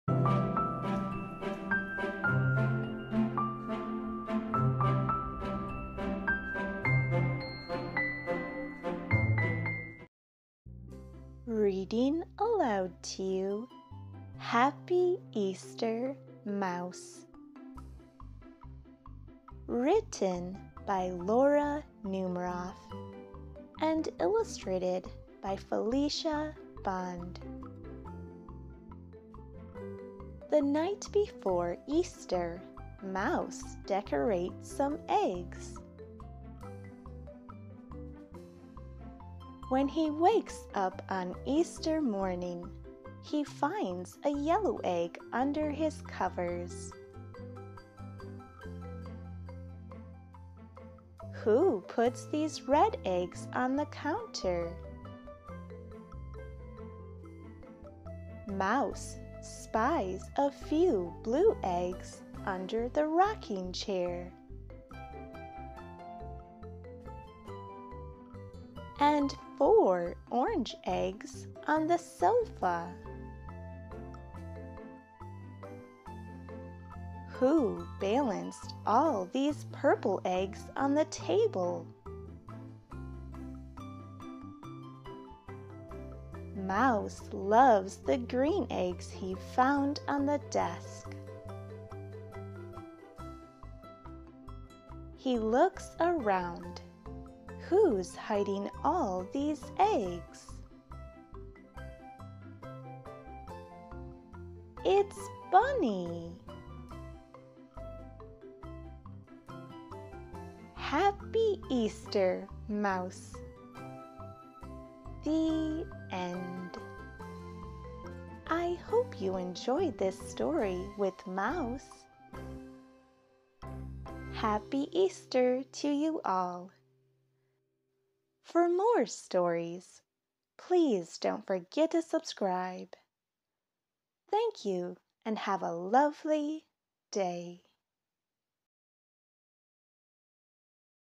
Happy Easter, Mouse! Storybook Read Aloud - Kids Easter Book Read Aloud
Reading aloud to you an Easter book, Happy Easter Mouse! Hope you enjoy this fun Easter story!

Happy Easter, Mouse!
Written by Laura Numeroff and Illustrated by Felicia Bond